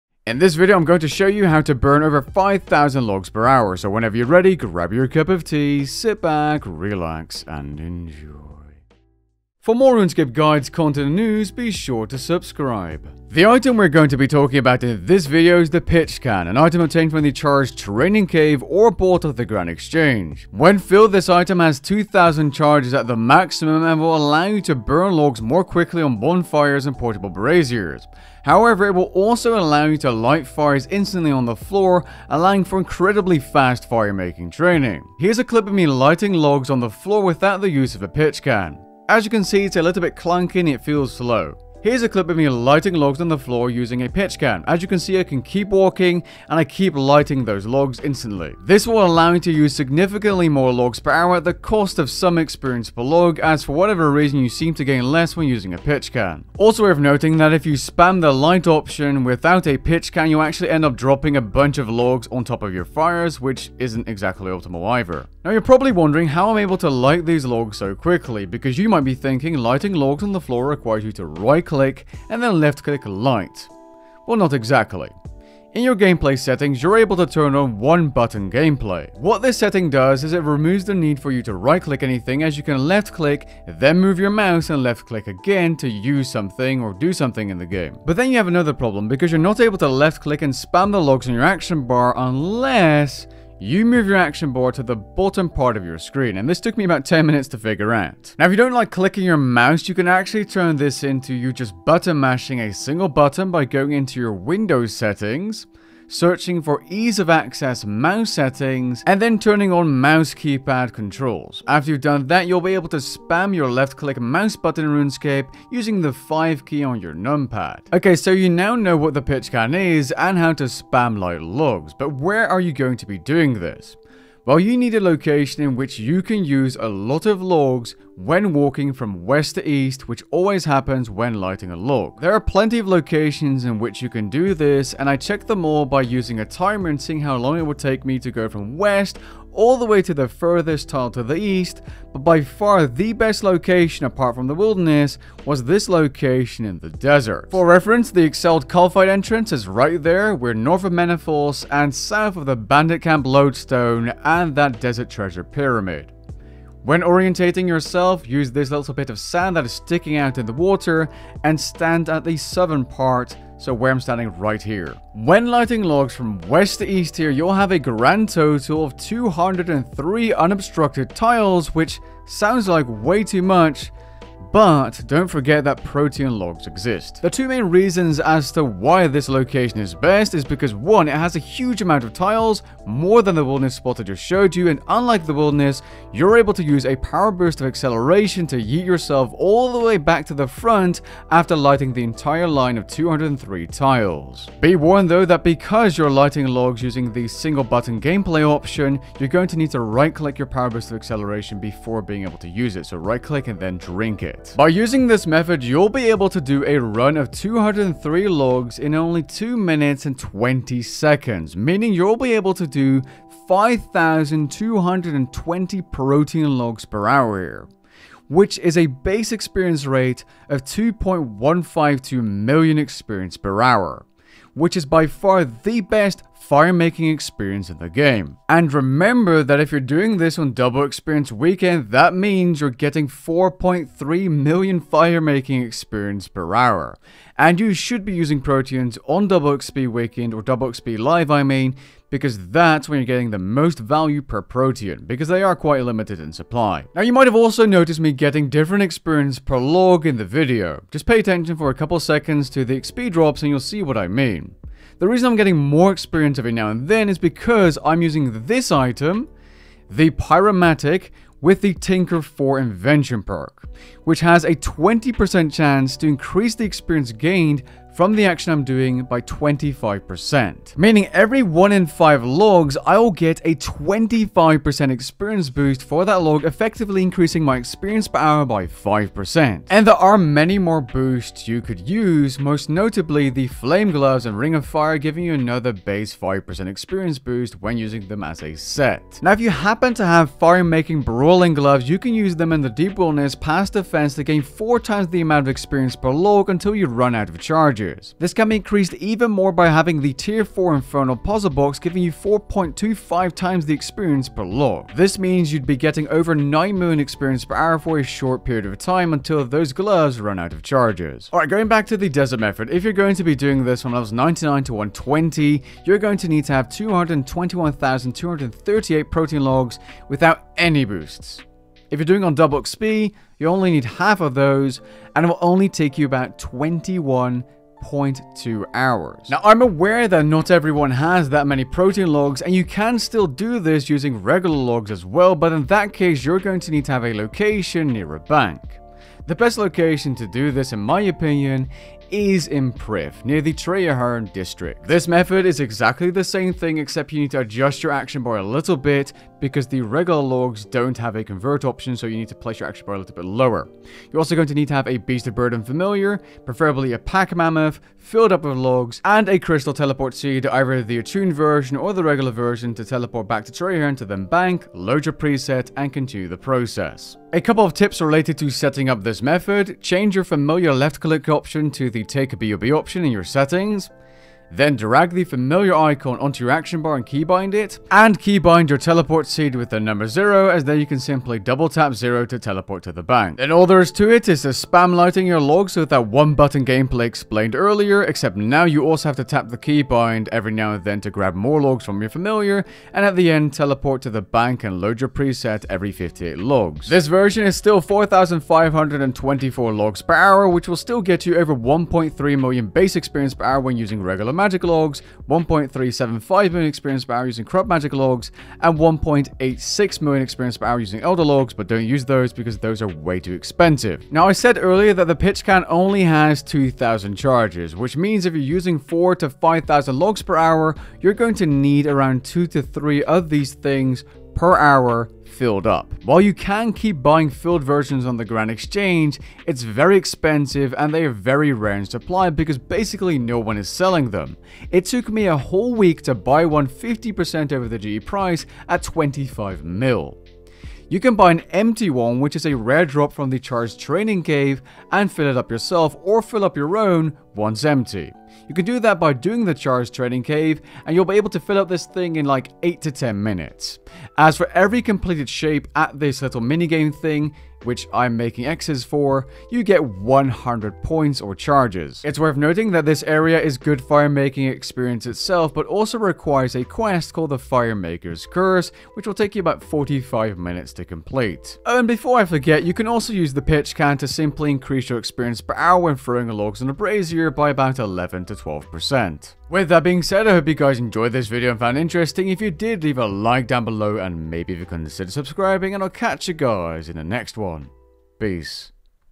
How to get 120 Firemaking in a single day
In this video I cover a somewhat unknown item that I rarely see players use or talk about. I believe the reason the item isn't talked about often is because players usually AFK Firemaking to 120 using a portable brazier, but even then it can be useful.

- Timestamps -
0:00 Intro
0:09 Subscribe
0:13 Pitch can
1:05 How to spam light logs
1:58 Where do you spam logs?
2:25 Optimal location using protean logs
4:07 Gain more XP
4:49 Firemaking brawlers
5:11 Optimal location using protean logs (continued)
5:29 Method without protean logs
6:55 Recharging the Pitch Can
7:53 Outro